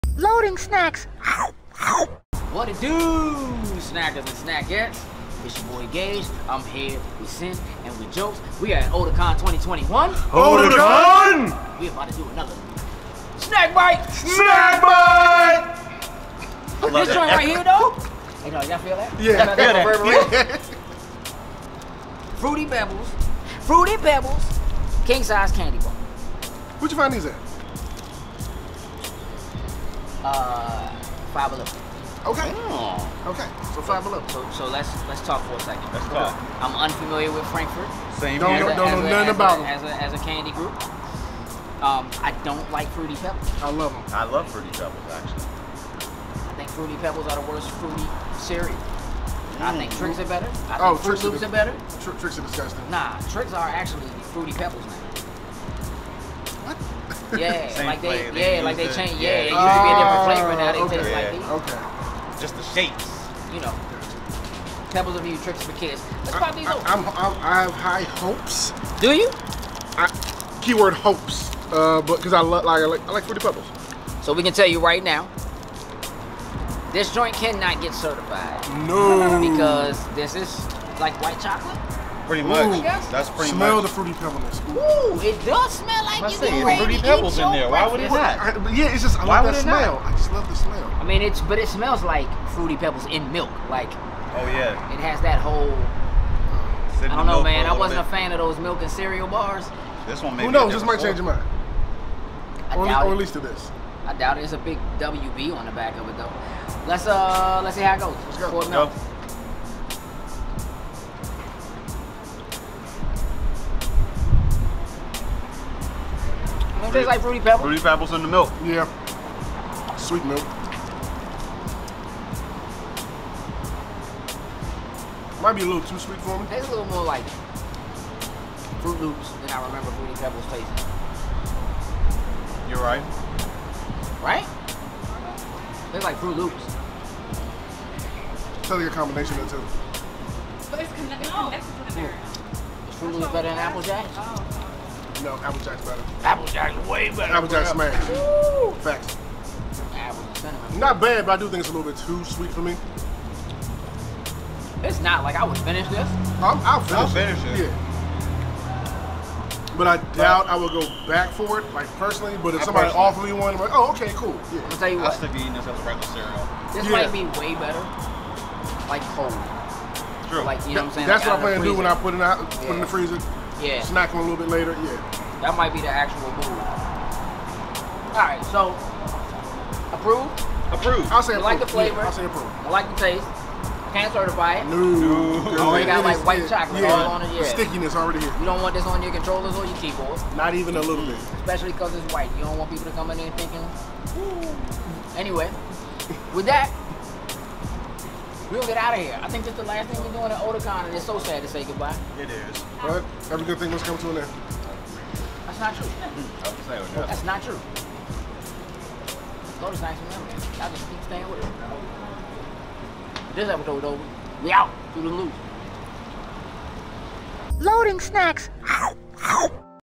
Fruity Pebbles Candy Bar SnackBite! | Otakon 2021
The LS Crew took a break during Otakon 2021 to step outside and try Fruity Pebbles Candy Bars. You read that right. FRUITY PEBBLES CANDY BARS!! Are they any good? Are they Loading Snacks Approved? Are they Loading Snacks Certified? Watch and find out.

Follow Loading Snacks on Twitter, Tiktok, & IG - @LoadingSnacks